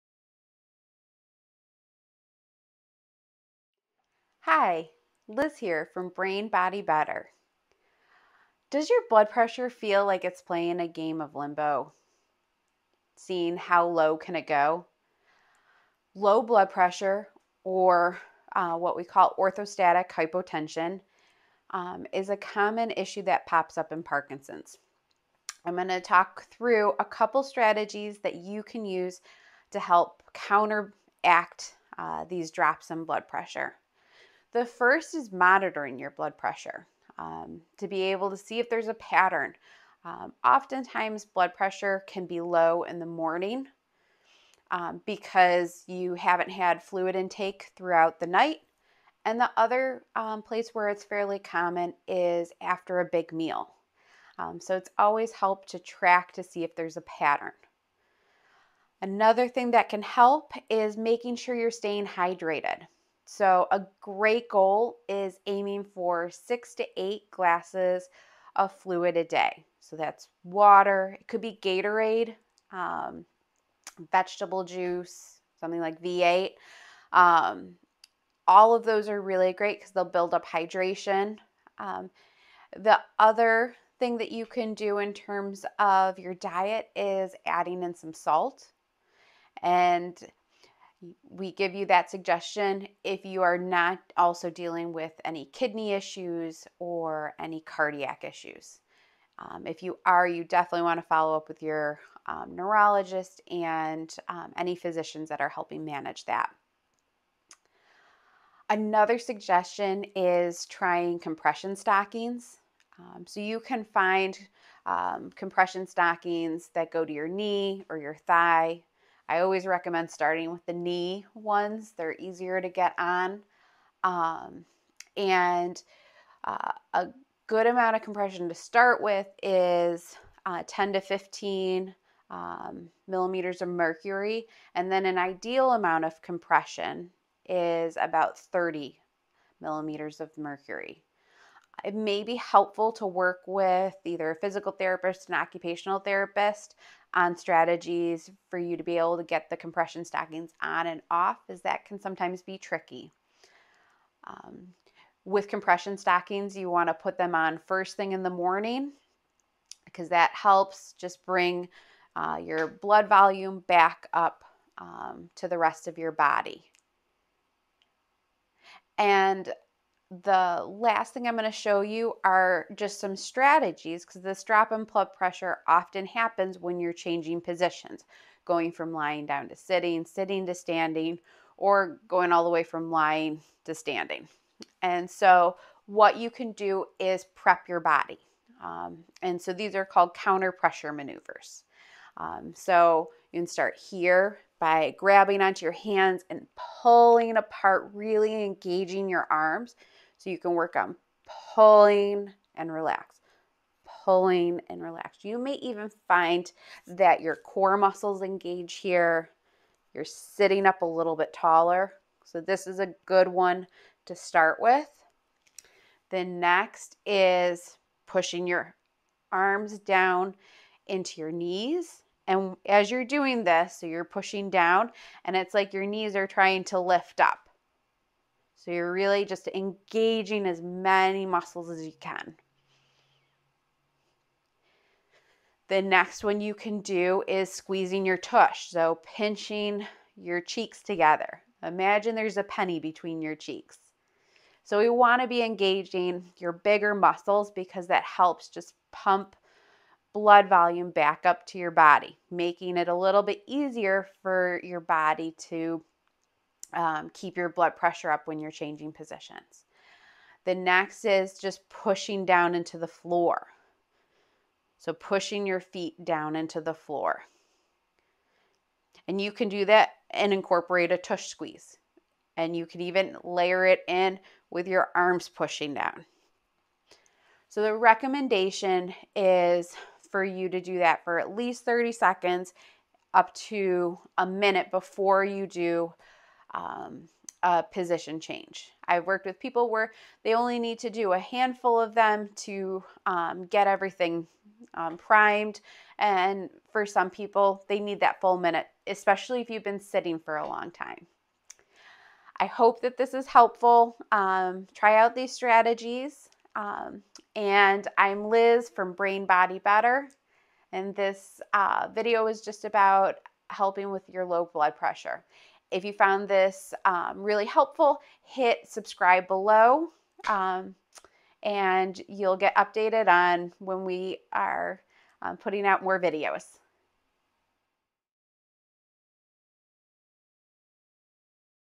How to combat Parkinson's low blood pressure
Liz goes over strategies to help you deal with low blood pressure (orthostatic hypotension) associated with PD.